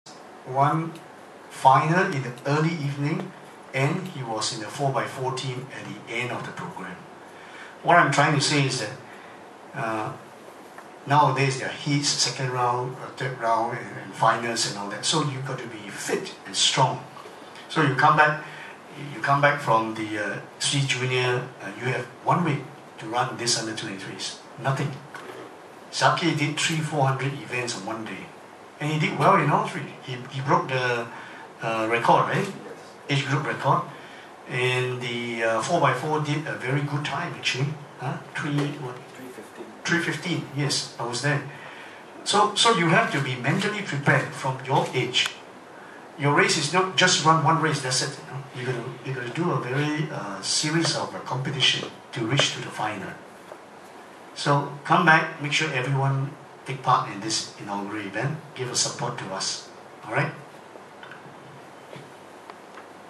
Mr. Tang Weng Fei highlights Zaki Sapari - 1st Singapore U23 / Open T&F C'ship Press Conference
SAA President Mr. Tang Weng Fei highlights Zaki Sapari feat at the recent Taiwan Open where he ran a record-breaking run twice in the 400m hurdles, and a final inspiring leg in the 4x400m to help the relay quartet to clock a season-best.

Mr. Tang rationale is that if junior athlete Zaki is able to perform remarkably thrice in one day, our juniors should have no issues participating in the inaugural 1st Singapore U23 / Open, after their overseas competition.